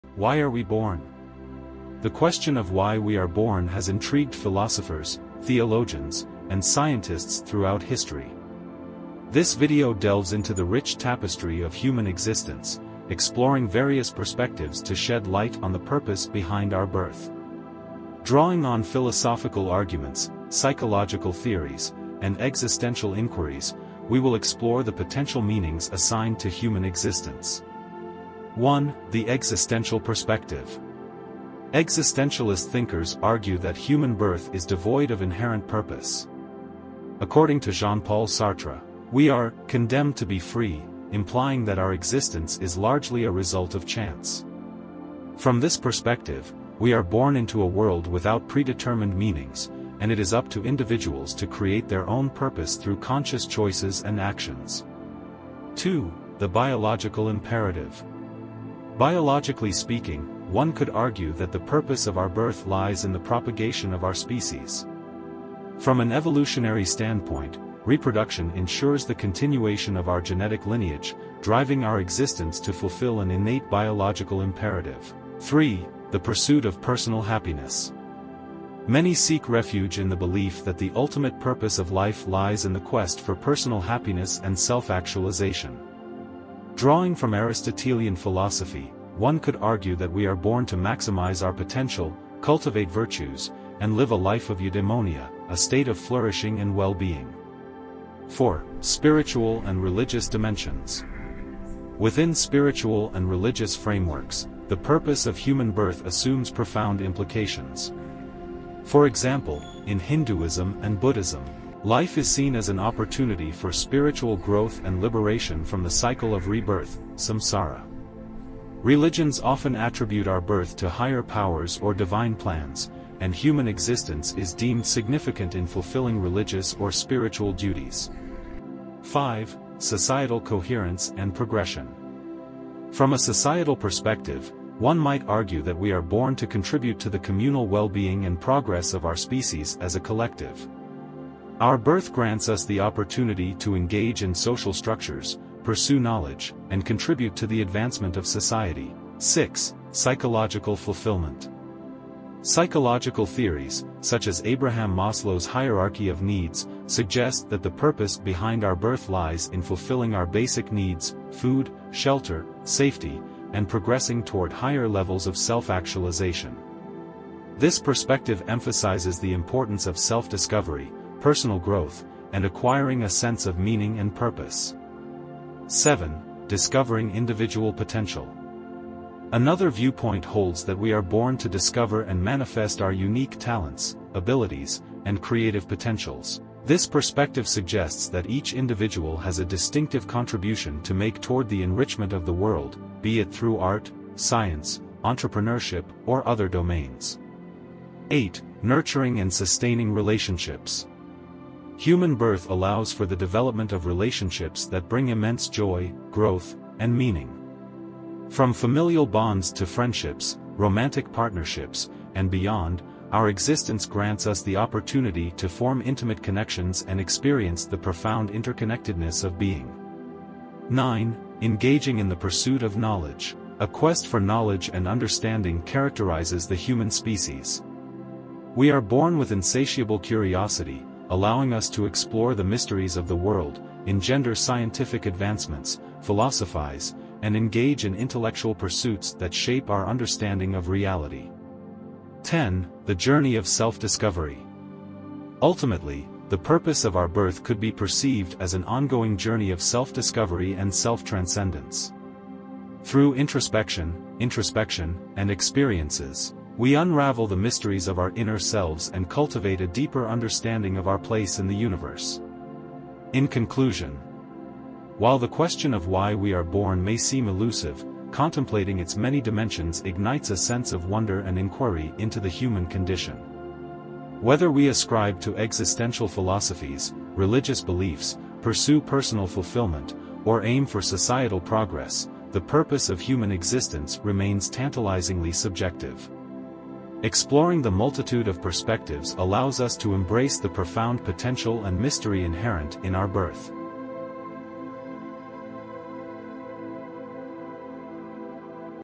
Why Are We Born
Introducing "Why Are We Born," a thought-provoking exploration into the meaning of our existence. This profound video delves into the fundamental question that has intrigued philosophers, scientists, and spiritual thinkers for centuries. With meticulous research and insightful analysis, this compelling masterpiece seeks to unveil the enigma of our purpose, guiding readers on a profound journey of self-discovery. Delivered in a clear and engaging writing style, "Why Are We Born" is a must-watch for those seeking to navigate life's mysteries with curiosity and introspection.